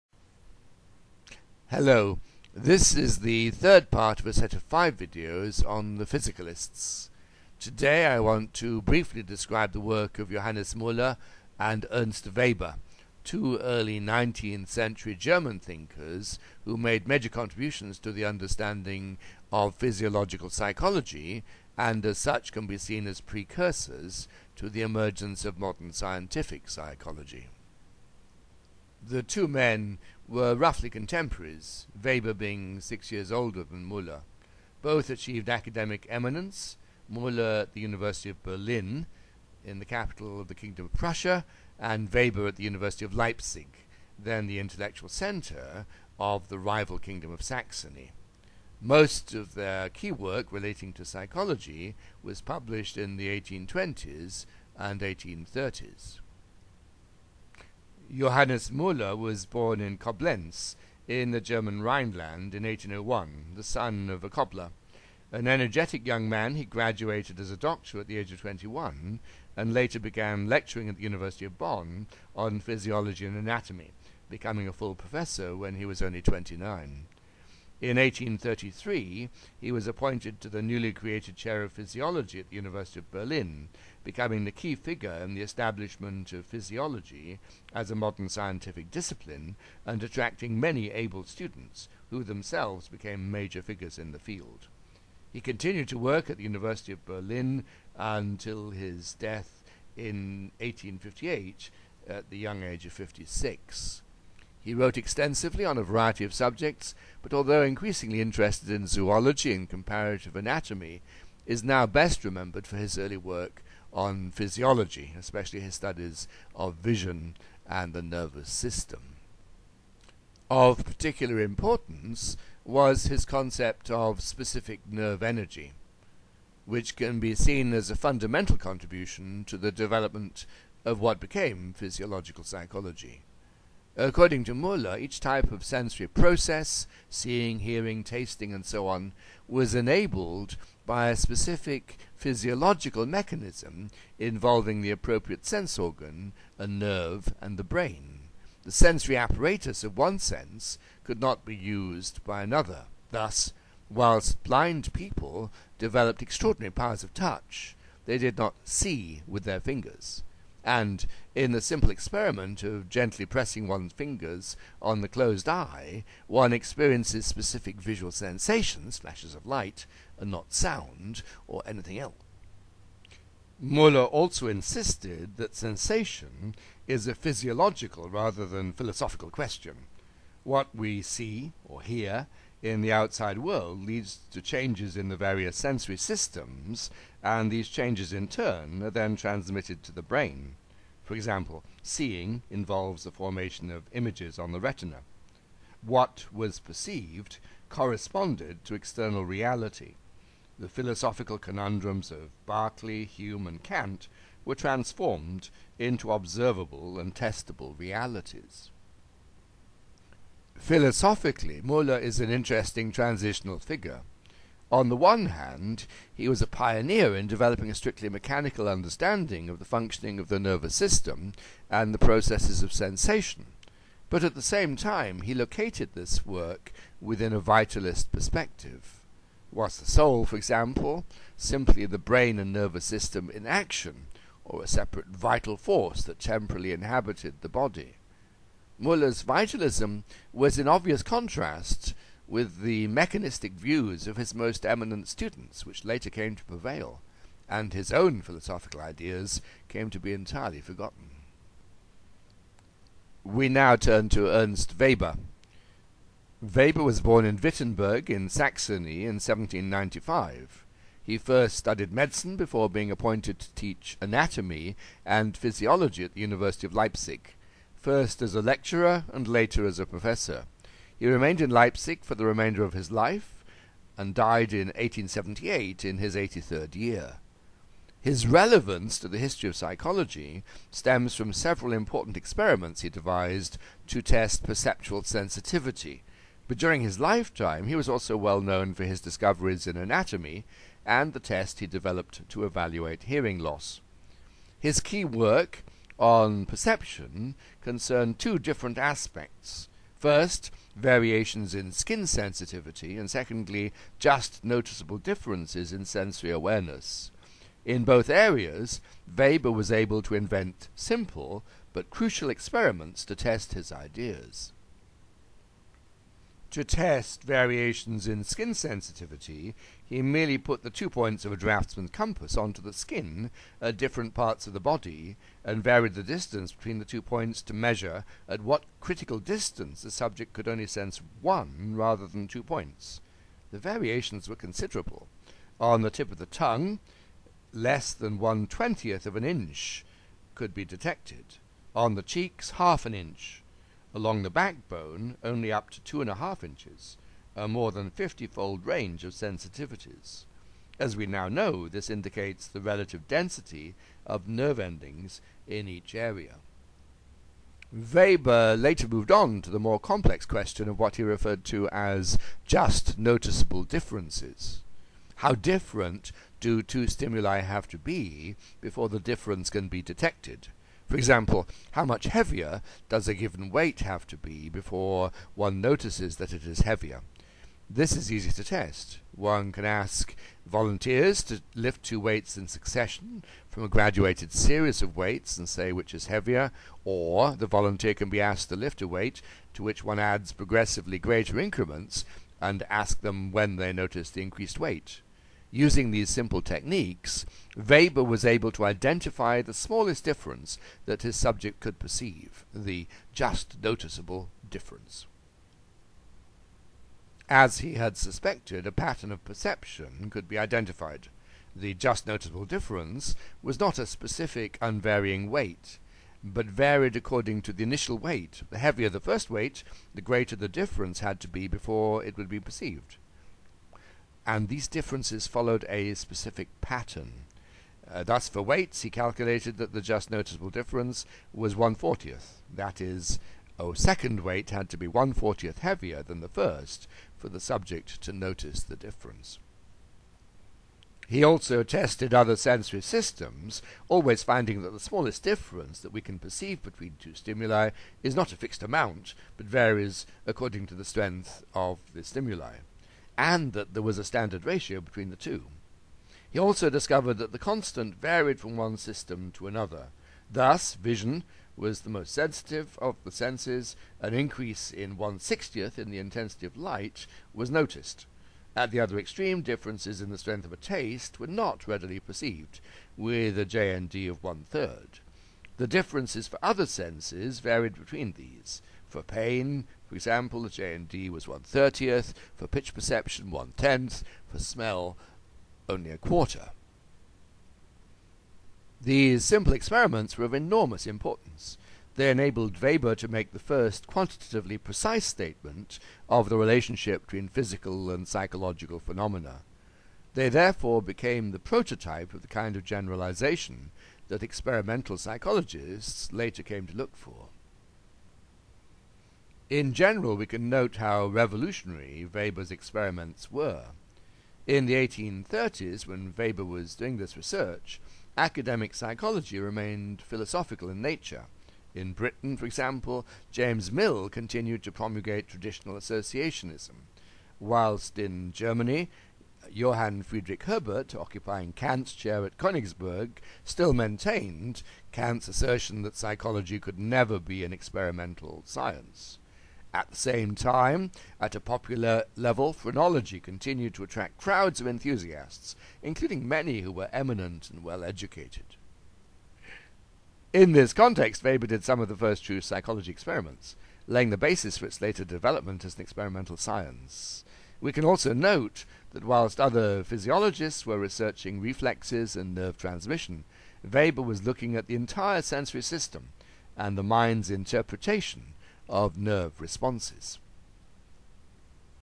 Johannes Muller and Ernst Weber [HPsy 4.3]
By Peter Smith. A brief description of the contributions of Muller and Weber to the development of physiological psychology.